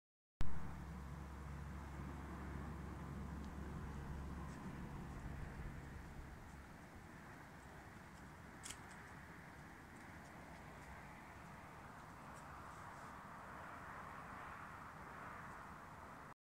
SPAIN - Night time view from our Villa El Cielo, Top Altea, Altea Hills
A birthday trip January 2020